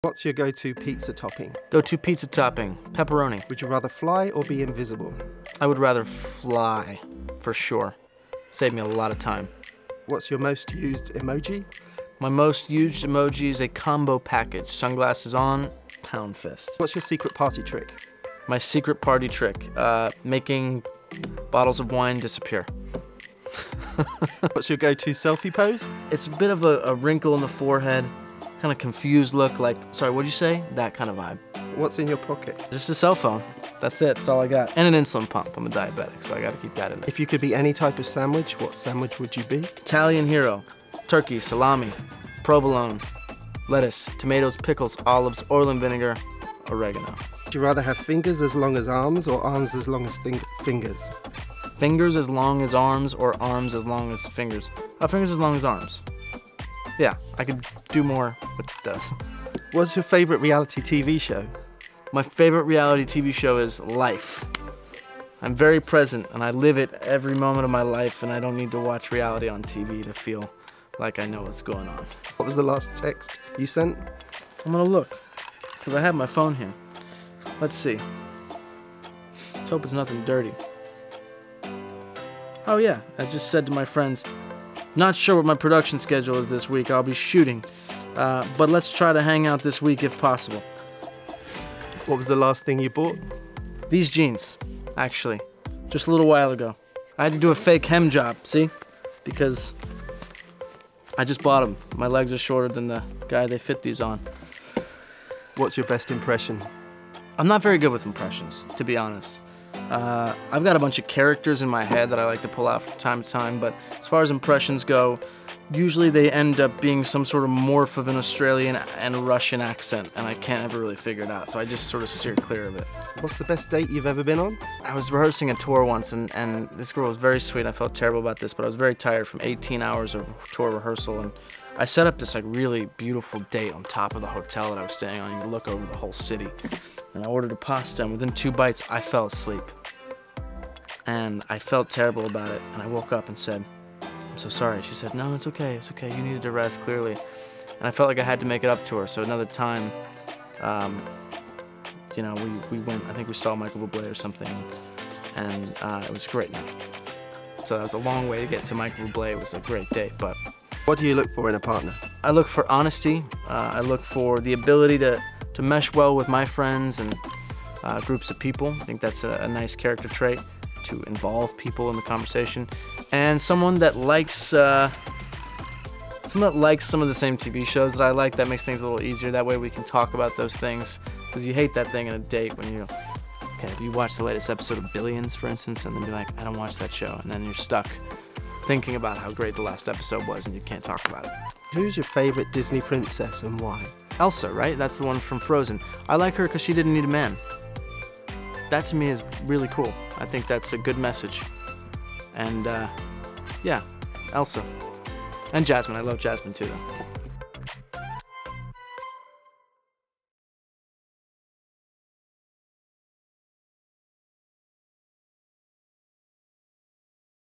Nick Jonas Goes Speed Dating! | MTV Movies
What’s the best date Nick Jonas has ever been on? What’s the last text he sent? And would he rather have fingers as long as arms or arms as long as fingers? ALL IS REVEALED as he takes the MTV Celebrity Speed Date!